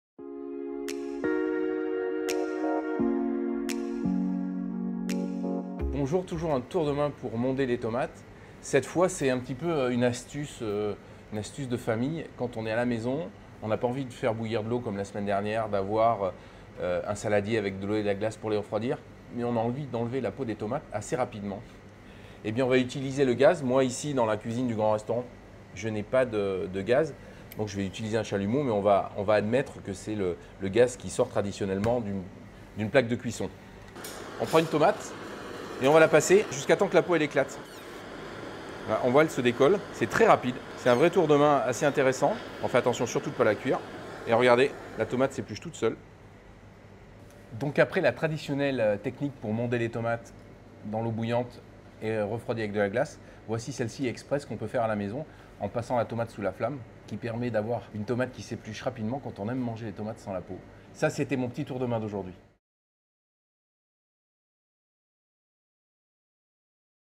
Tombez dans le Piège #181 : monder les tomates à la flamme
Le chef 2 étoiles du Grand Restaurant à Paris vous confie chaque samedi ses secrets.